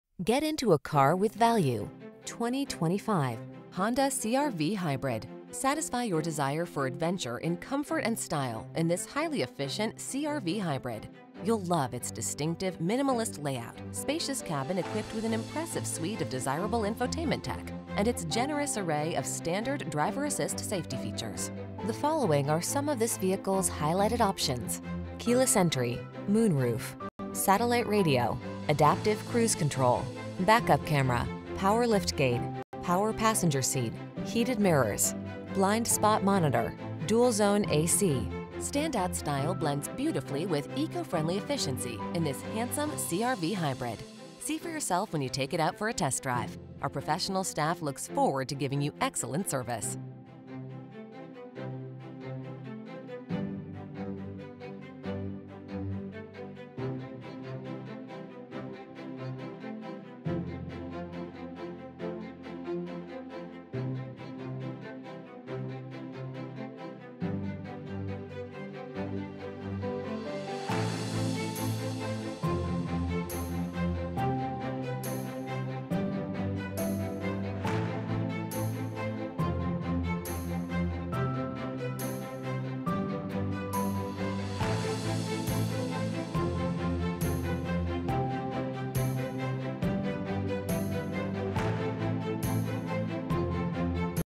2025 Honda CR-V_Hybrid Sport-L Aiken, North Augusta, Augusta, Evans, Grovetown SC
New 2025 Honda CR-V_Hybrid Sport-L available at Honda Cars of Aiken located in 550 Jefferson-Davis Hwy, Warrenville, SC, 29851. Servicing the Aiken, North Augusta, Augusta, Evans, Grovetown area.

CR-V_Hybrid

Get into a car with value, the   2025  Honda CR-V_Hybrid. Satisfy your desire for adventure in comfort and style in this highly efficient CR-V Hybrid. You'll love its distinctive, minimalist layout, spacious cabin equipped with an impressive suite of desirable infotainment tech, and its generous array of standard driver-assist safety features.  The following are some of this vehicles highlighted options: Keyless Entry, Moonroof, Satellite Radio, Adaptive Cruise Control, Back-Up Camera, Power Liftgate, Power Passenger Seat, Heated Mirrors, Blind Spot Monitor, Dual Zone A/C. Standout style blends beautifully with eco-friendly efficiency in this handsome CR-V Hybrid. See for yourself when you take it out for a test drive. Our professional staff looks forward to giving you excellent service!